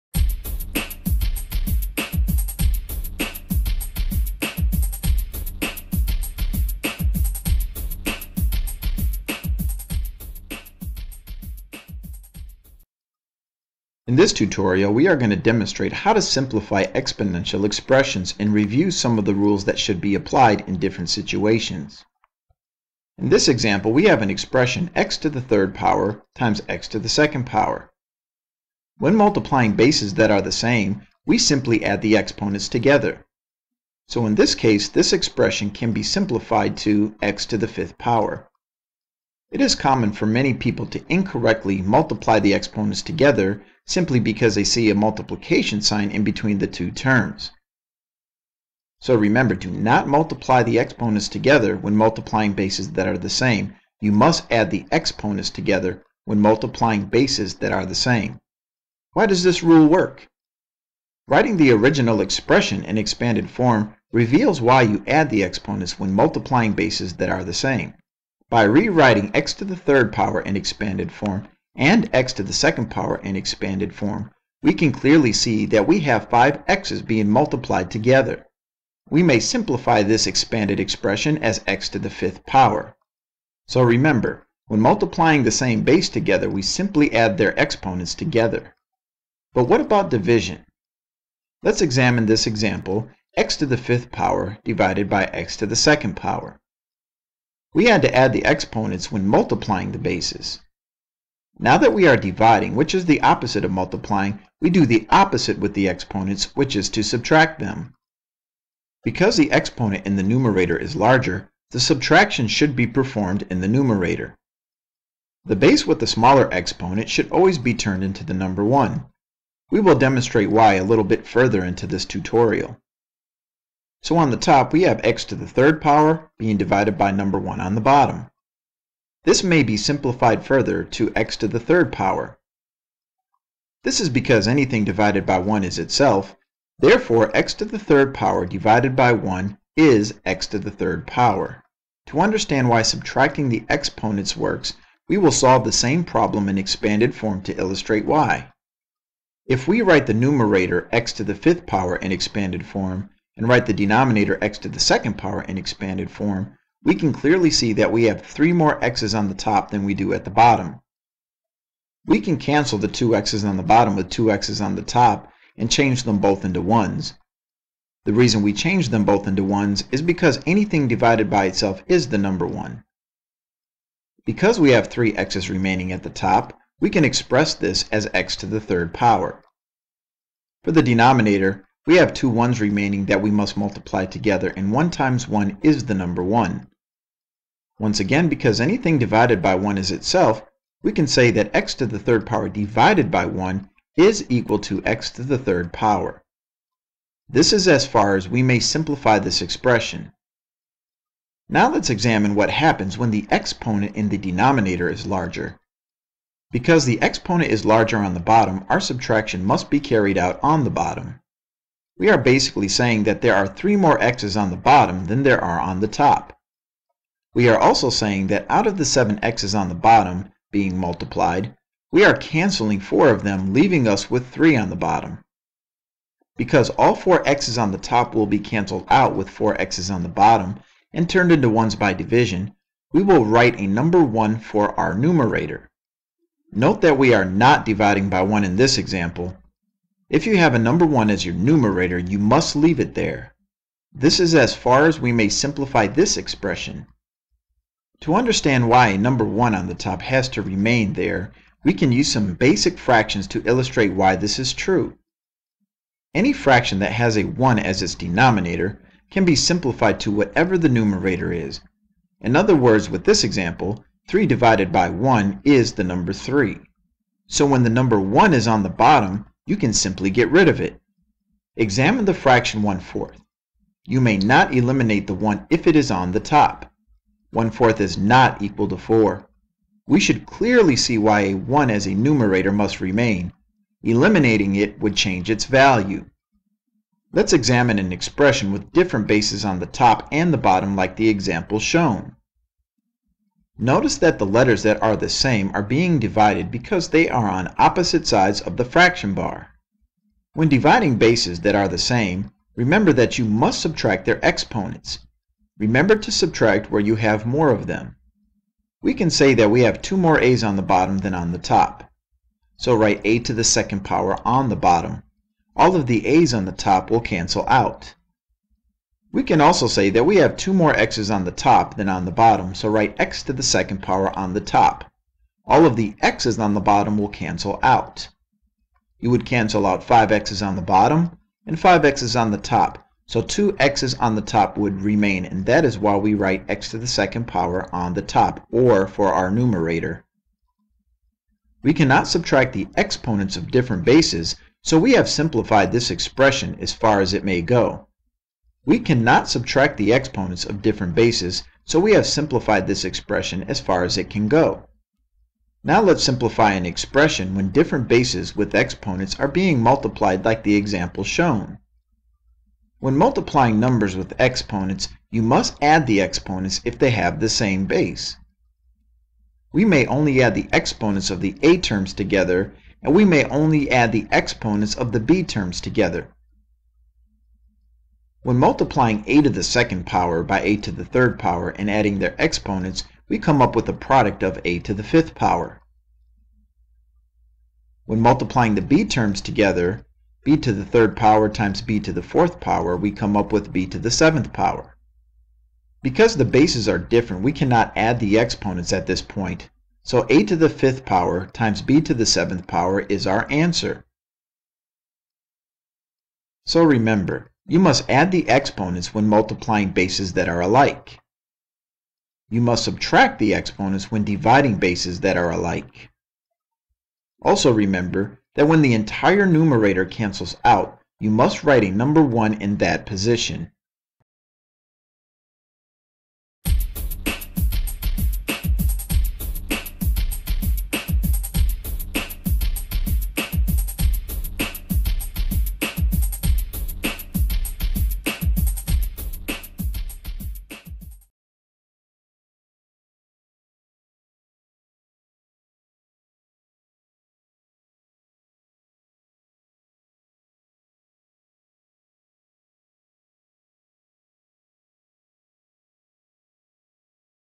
Simplifying Exponential Expressions
This video reviews how to simplify expressions containing exponents.  Learn when you should add the exponents and when you should subtract them.